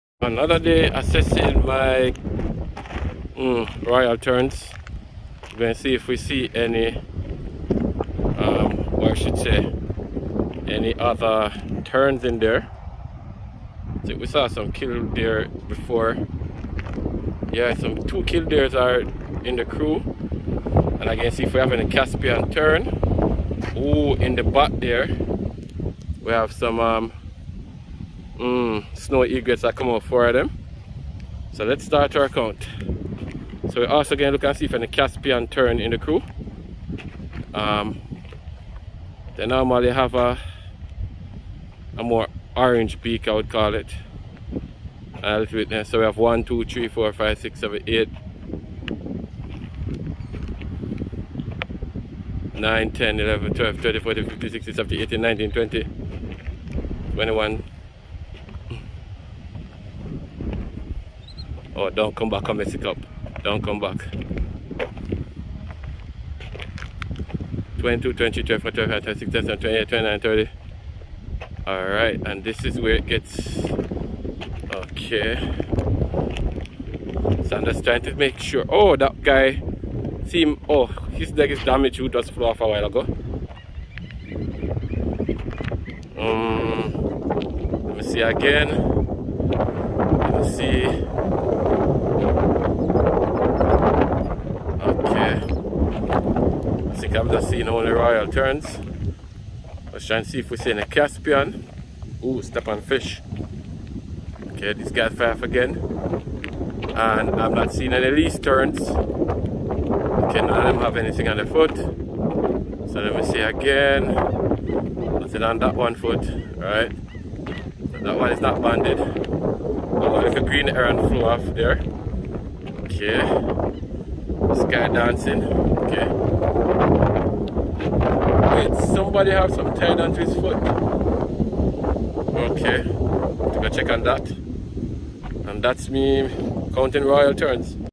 Counting Royal Terns at Sewage Plant
Part1: attempting to count the Royal Terns at the Hellshire Sewage Plant.

Did you know that a number of these birds migrate from North America to the Caribbean to escape the winter  ?

They be doing transatlantic vacation long before humans could do it.